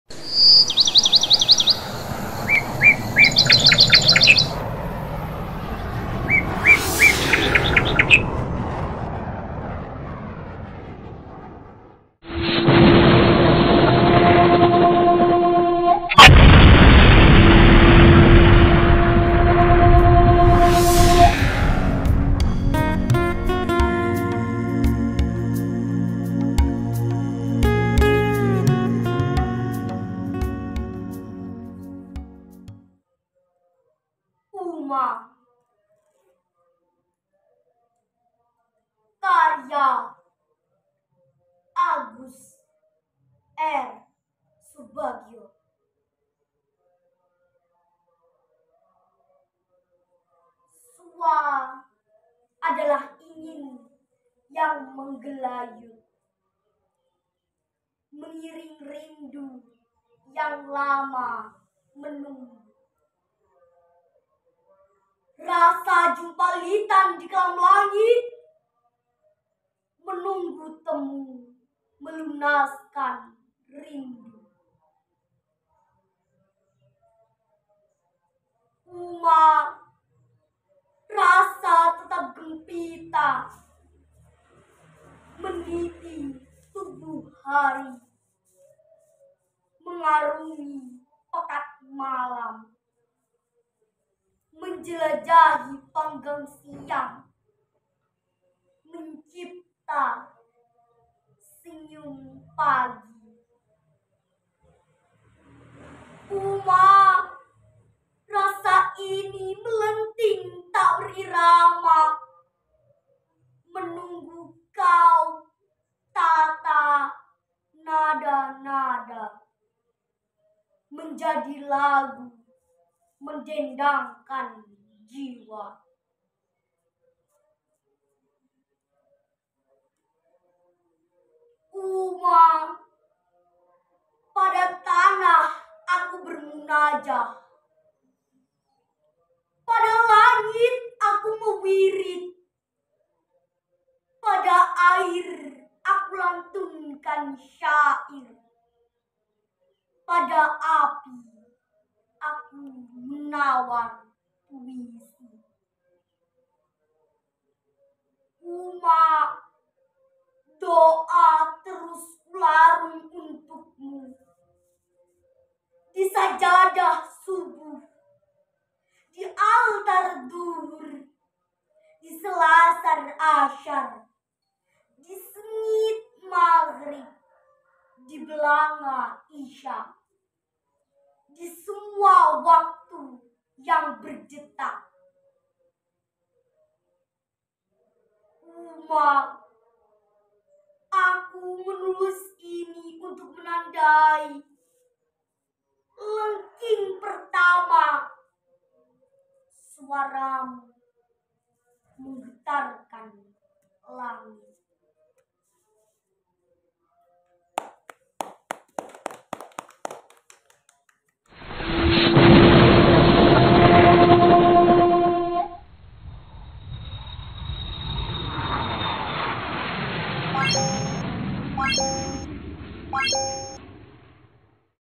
Uma - Agus R. Subagyo | Ilmira Lathifa - MI Darul Muta'allimin Cepoko, 06-03-2022 a
Ilmira Lathifa
Kelas 4
MI Darul Muta'allimin Cepoko Berbek Nganjuk

Sanggar Rumah Ilalang
Komunitas Seni Ilalang Indonesia

Baca Puisi
Uma
Karya: Agus R Subagyo

Dok. Latihan Rumah Ilalang, 06 Maret 2022 a